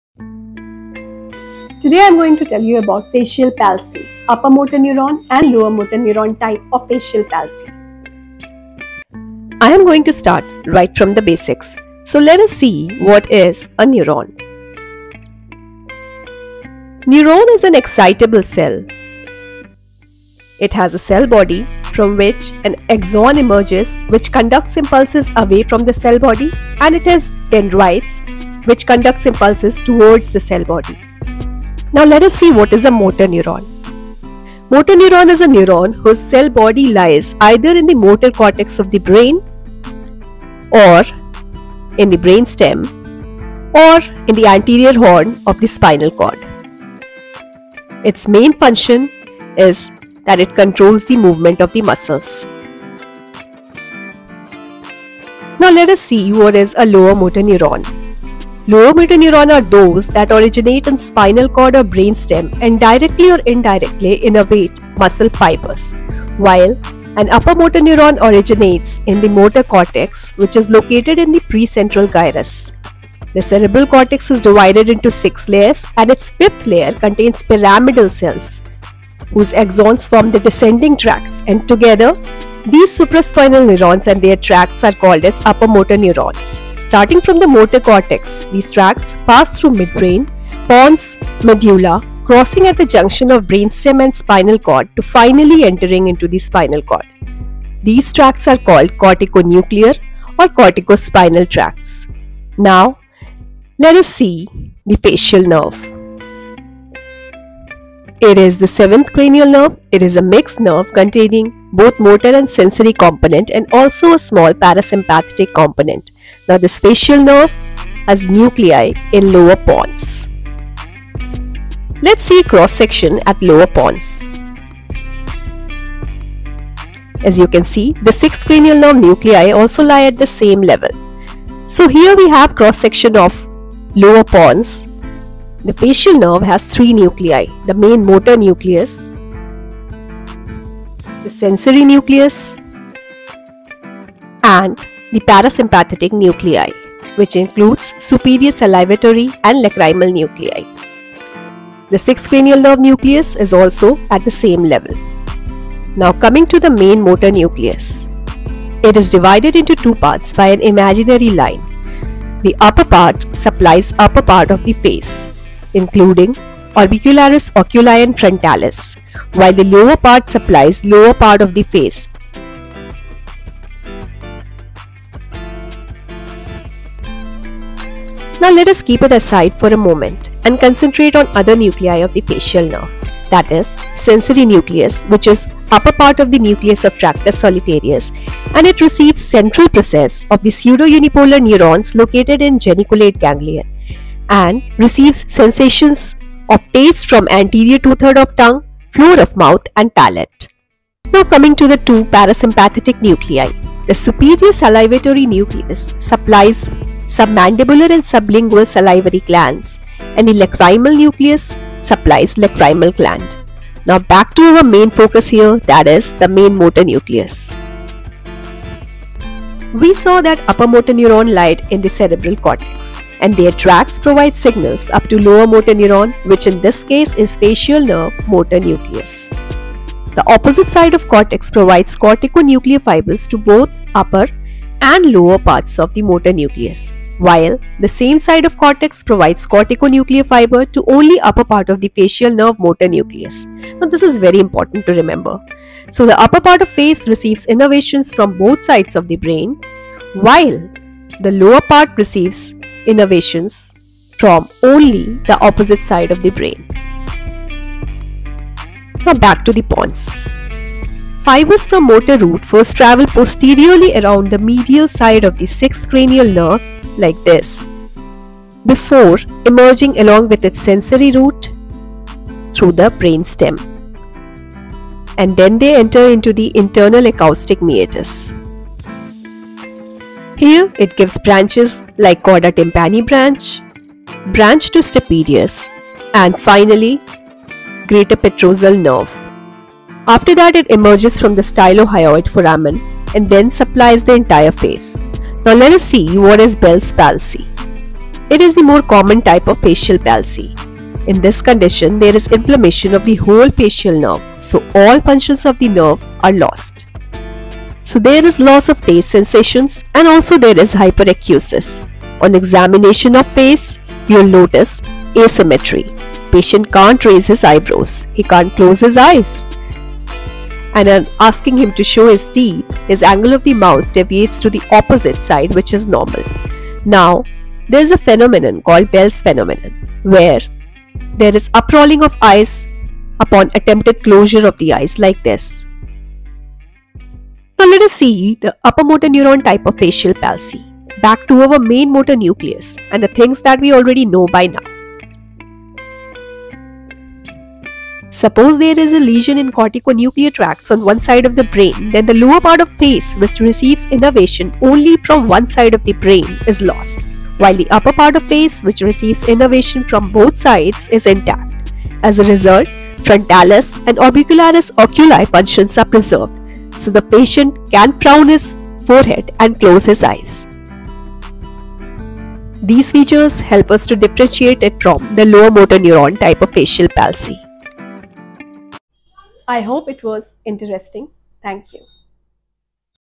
What is Upper Motor and Lower Motor Neuron type of Facial Nerve Palsy? Bell’s Palsy.
Here you will find the basic concept of Upper motor Neuron and lower motor Neuron type of lesions, especially regarding facial nerve. How to differentiate Bell’s Palsy from the other lesions.